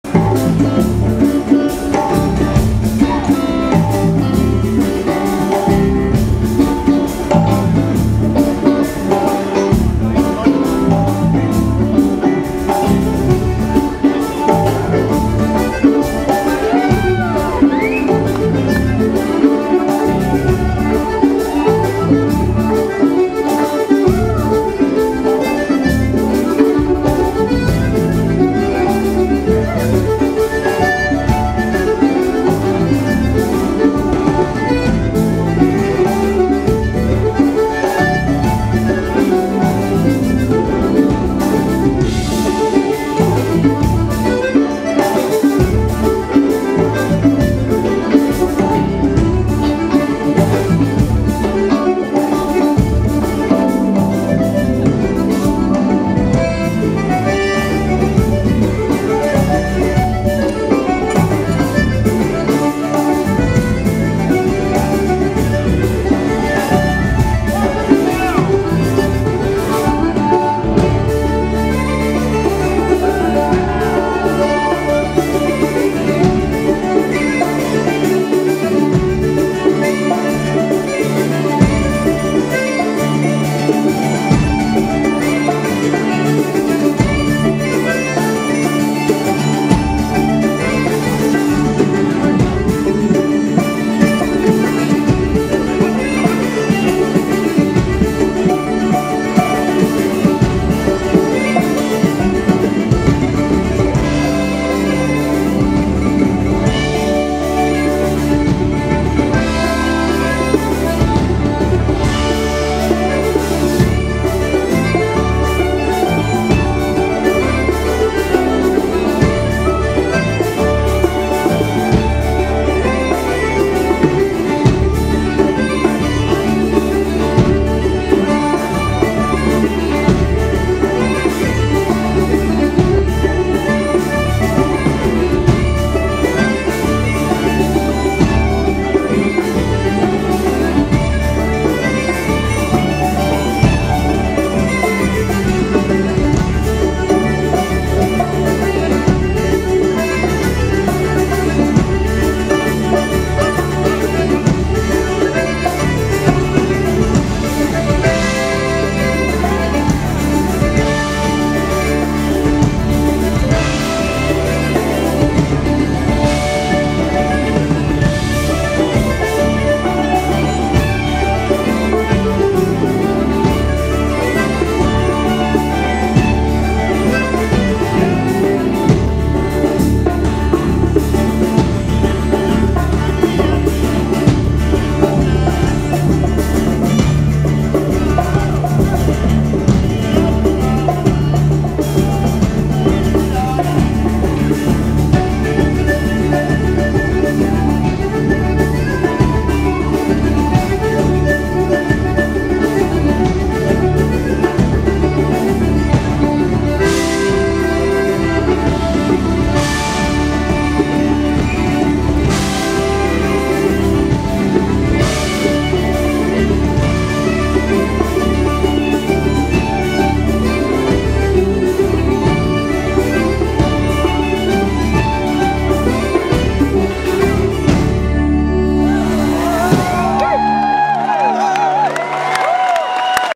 Moving Hearts with Máirtín O'Connor
Moving Hearts with special guest Máirtín O'Connor at Killarney Folkfest in the I N E C July 24 2015